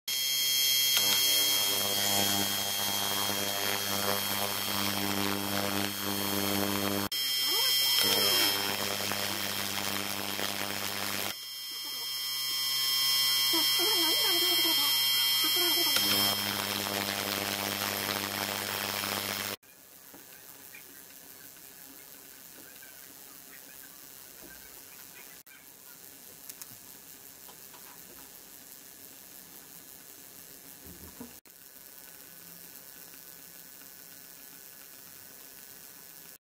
小型・脱気・マイクロバブル発生液循環（supersonic wave）
（超音波制御技術　Ultrasonic Sound Flow water effect）

水槽を含めた、各種容器の音響特性・液循環の効果を利用します。

　の適応技術として利用しています。

　音圧測定解析により確認しています